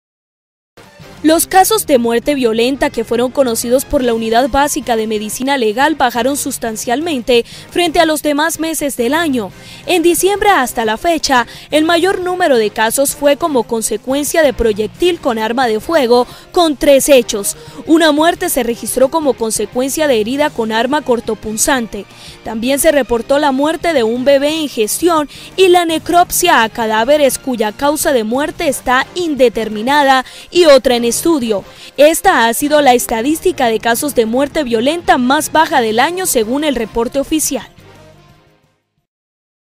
Ocho casos de muertes fueron conocidos por Medicina Legal en Diciembre
Jueves 27 de Diciembre de 2012